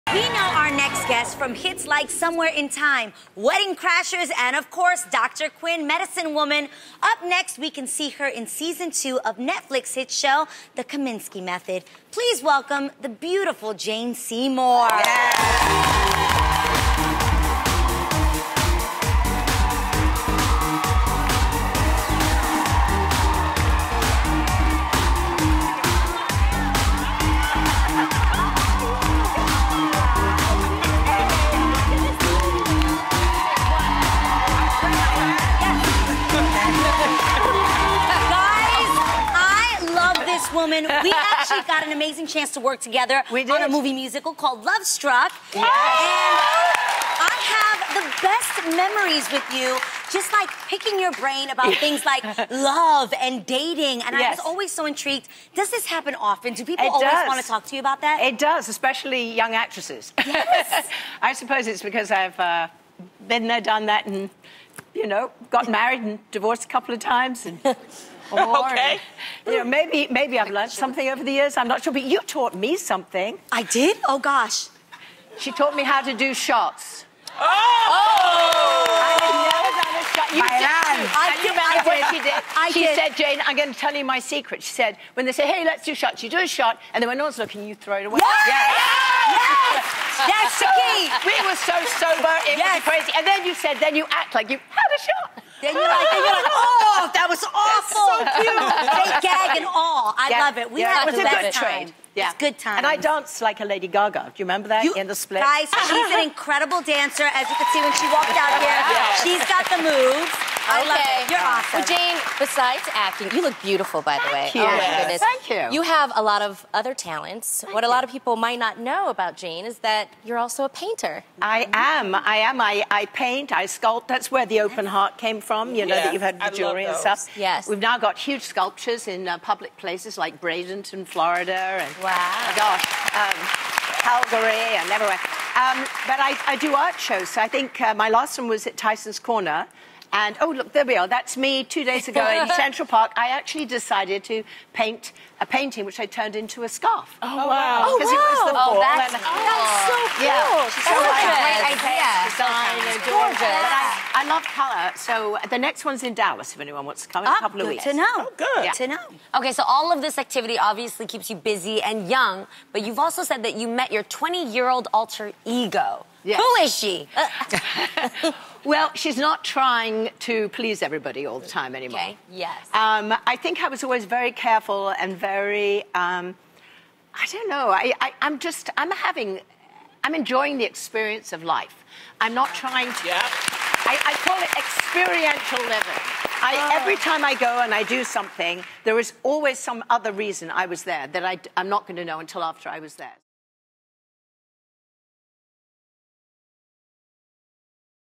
FULL INTERVIEW PART ONE: Jane Seymour on “The Kominsky Method” and More!
Real fam, what’s your current Netflix binge-watch?